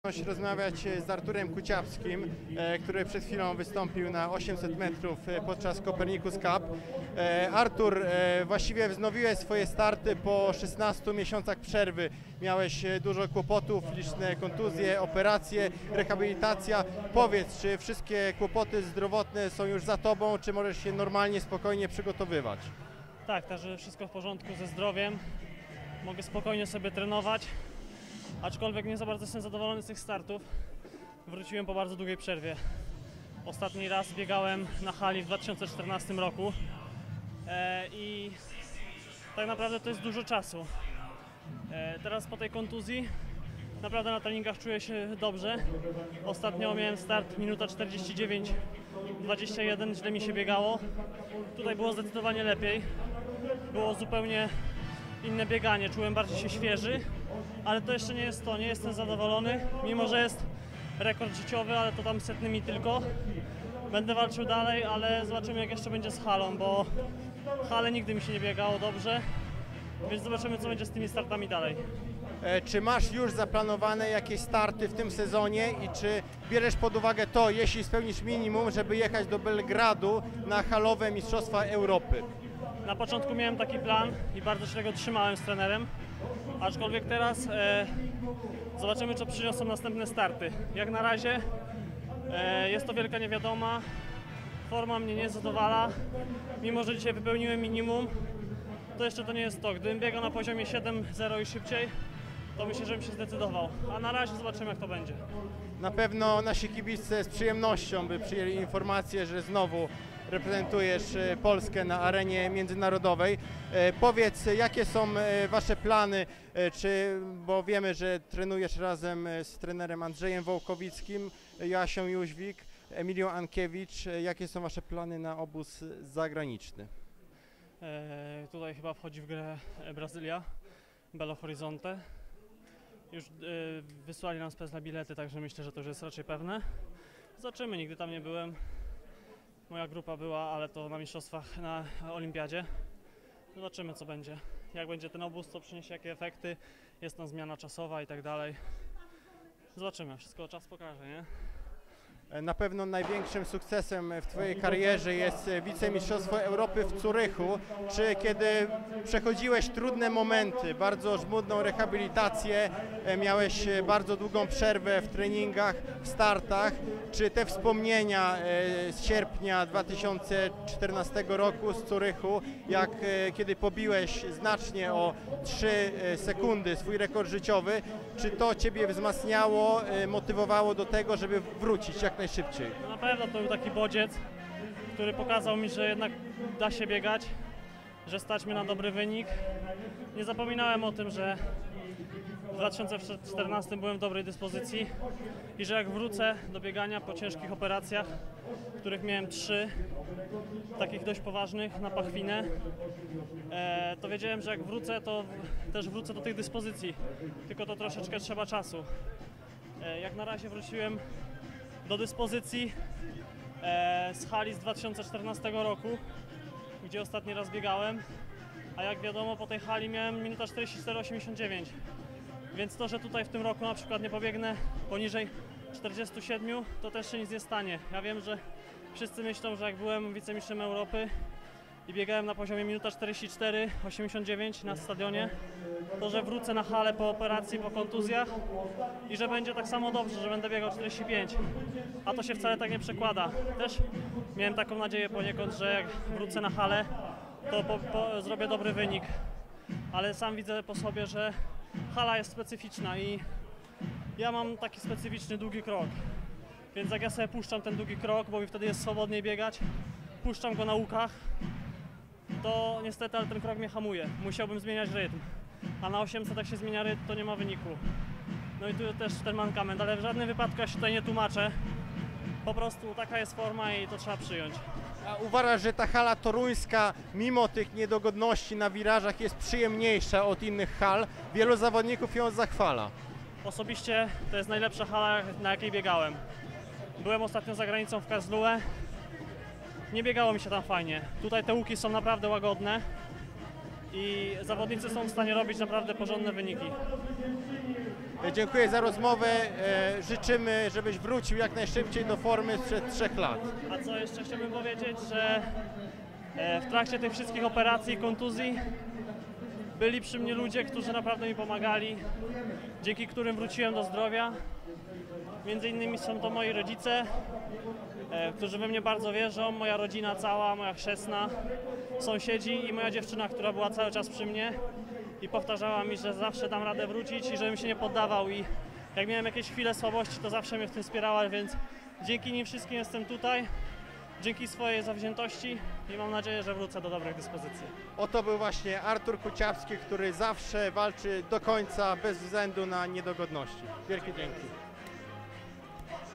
Artur Kuciapski: "Dzięki swoim bliskim i zawziętości jestem tutaj"
Artur Kuciapski, wicemistrz Europy z Zurychu (2014). Walczy o powrót do formy po kontuzji.